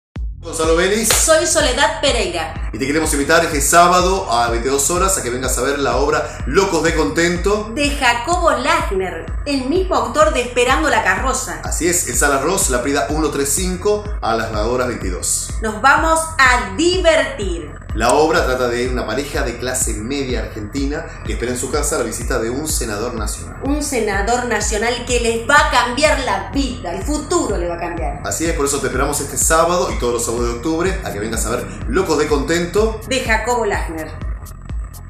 Este sábado vuelve a la Sala Ross “Loscos de Contentos”
Vuelven “Locos de Contentos”, una comedia para disfrutar a pleno. A partir de las 22, en la Sala Ross, Soledad Pereyra y Gonzalo Veliz proponen situaciones extremas que van y vienen.
Este sábado a partir de las 22, en Laprida 135, la Sala Ross proyectará “Locos de Contentos”, dirigida por Viviana Pera y Manuel Villarubia.
La comedia aborda la historia de un matrimonio joven: Claudia y Román, acosados por los problemas y las costumbres del país se resisten a perder las esperanzas y se proponen a recibir en su casa a un senador de la Nación. Todo está preparado para que cumplan su objetivo de conquistar al senador y conseguir un cargo diplomático para Román en el exterior y "vivir unos años afuera".